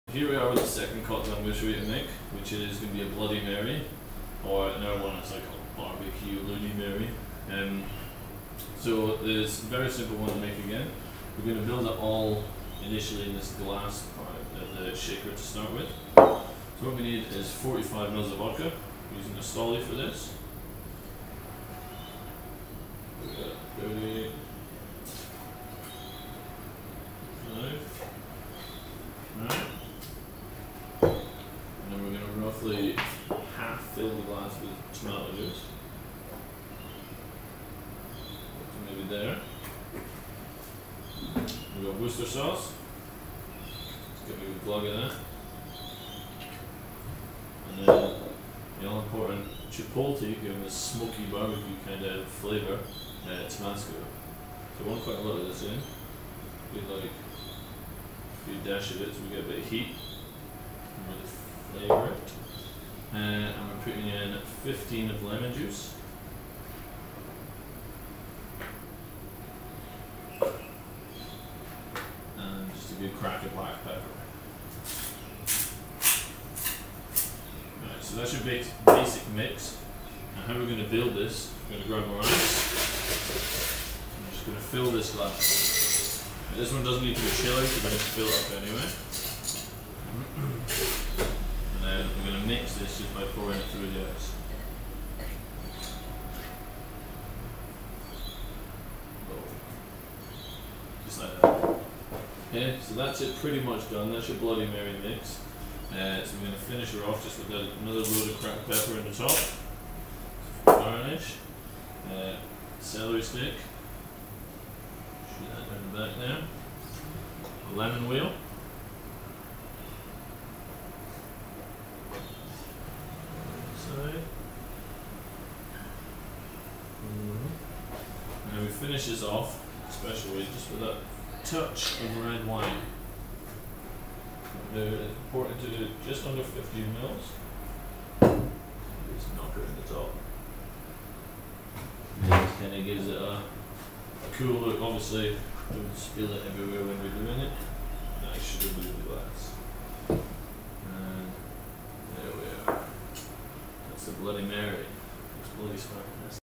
The Old Fitz BBQloody Mary
The BBQloody Mary

• Bloody Mary –
• 45ml Stoli Vodka,
• 150ml Tomato Juice,
• 6-8 dashes Chipotle Tabasco,
• splash Worcester Sauce,
• 15ml Lemon Juice,
• Pepper, 15ml Red wine.
1. Chill Glass
2. Add Vodka to glass shaker
3. Half fill with tomato juice
4. Add tabasco, Worcester and Lemon Juice
5. Few twists of Pepper
6. Pour contents into Ice Filled, Chilled Glass
7. Garnish with Pepper Lemon wheel and Celery
8. Finish with Red Wine

Easy.